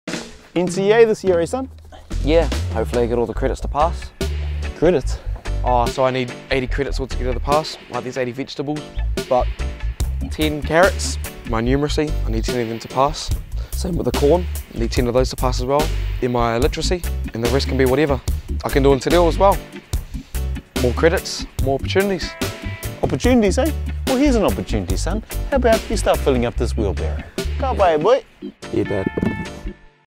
NCEA: More credits, more opportunities
What’s NCEA all about? How do I help my son or daughter pass? Check it out!